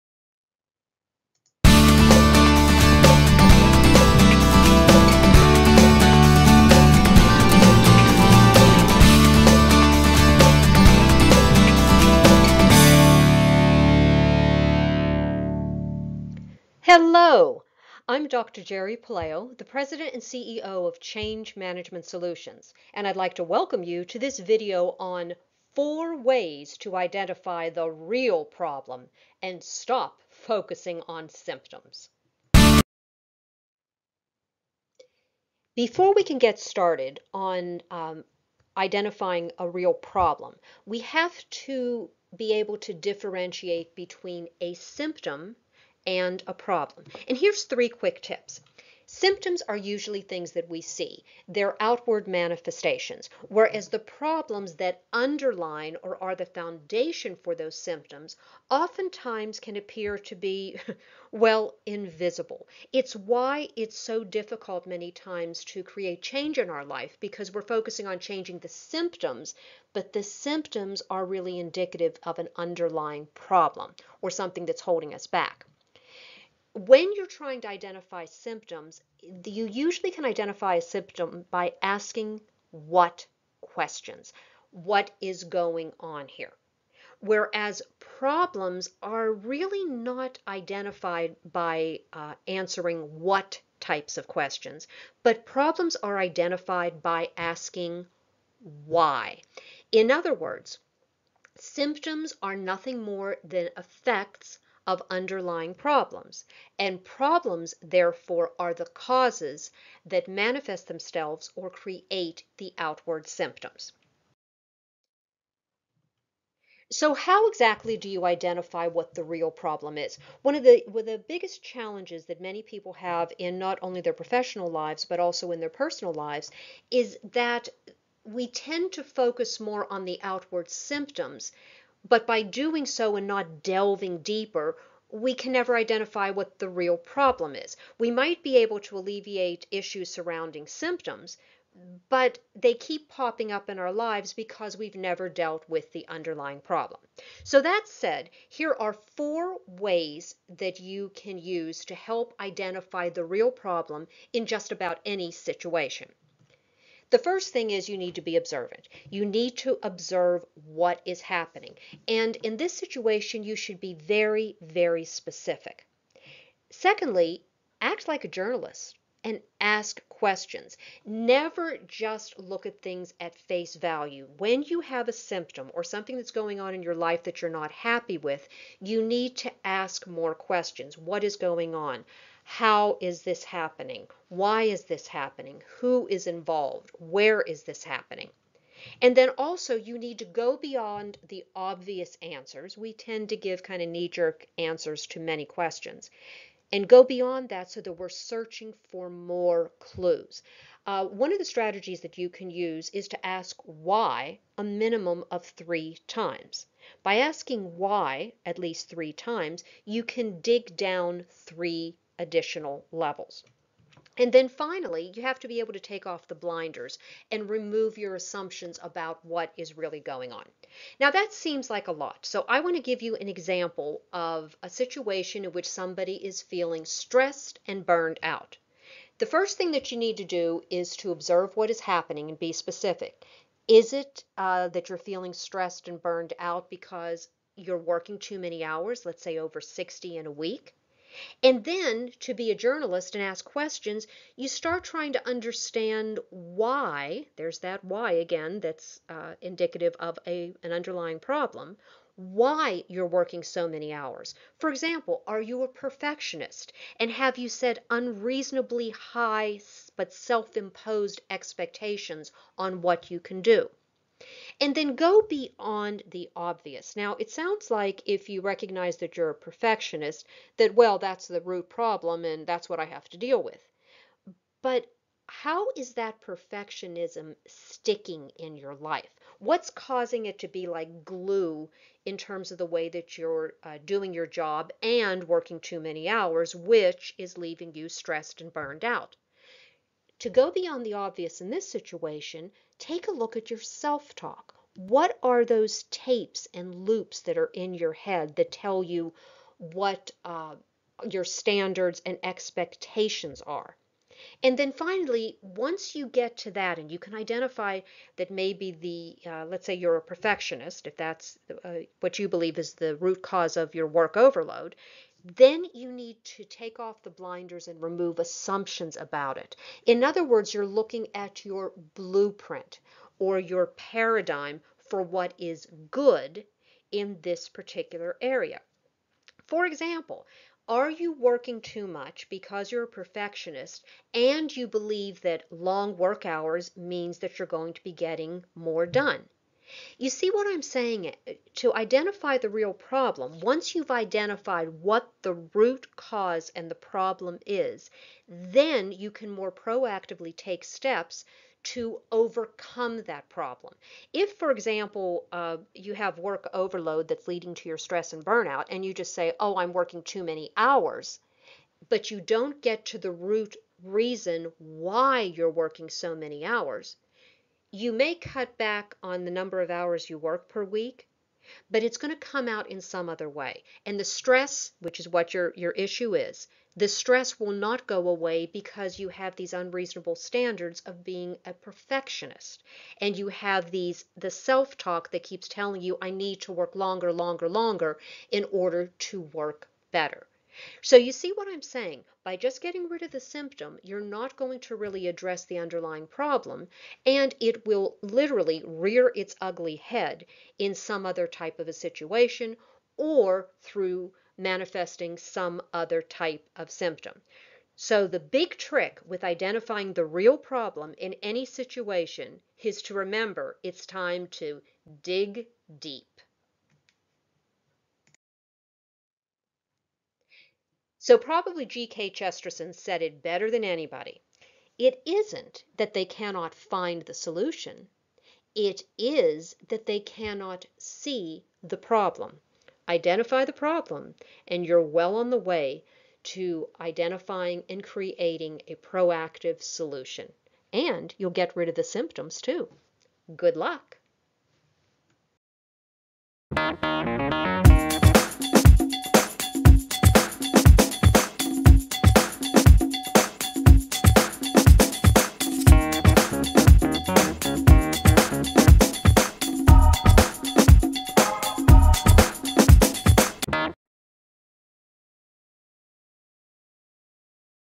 4 Ways to Identify the Real Problem and Stop Focusing on Symptoms Dr Geri Puleo 2018
Are you stuck in a rut?  Maybe you've been focusing too much on the symptoms, but haven't yet been able to identify the underlying problem.  Dr. Geri Puleo (creator of the Burnout During Organizational Change Model, "B-DOC") shares 4 ways to help you identify the real problem so that you can take proactive steps to overcome the challenge and remove the symptoms that have been holding you back.